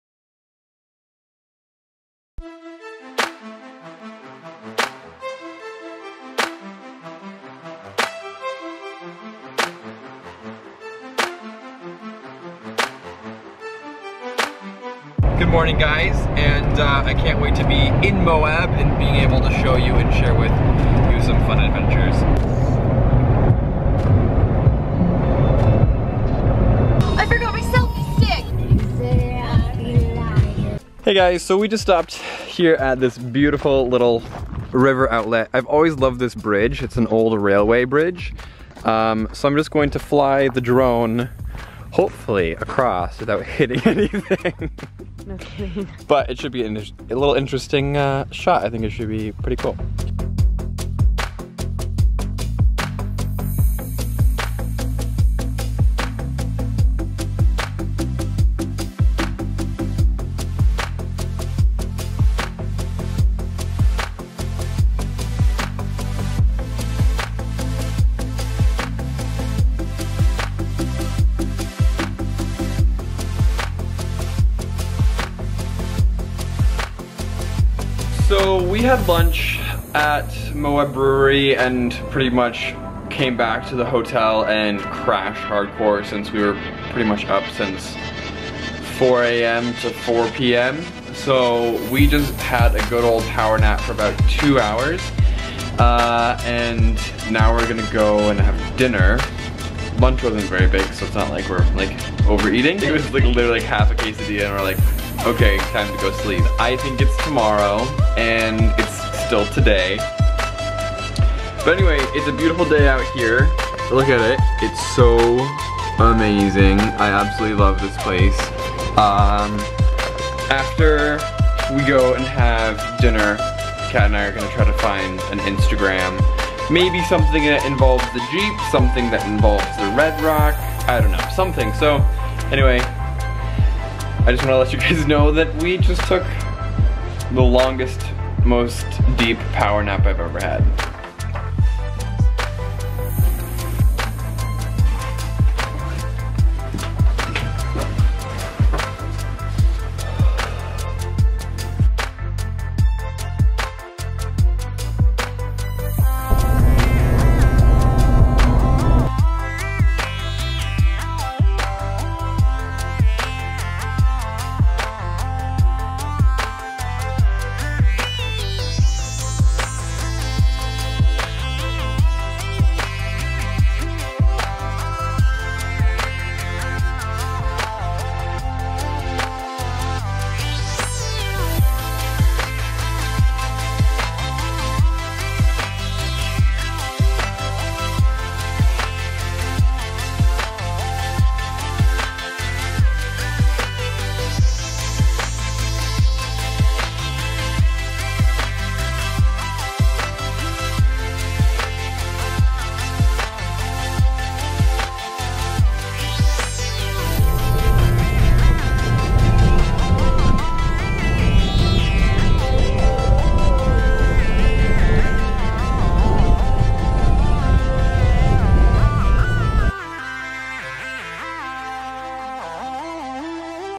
Over a Broken Bridge Moab Utah
This is the trip out and the first day I was in Moab. This was the first time I had gotten to go to Easter Jeep Safari.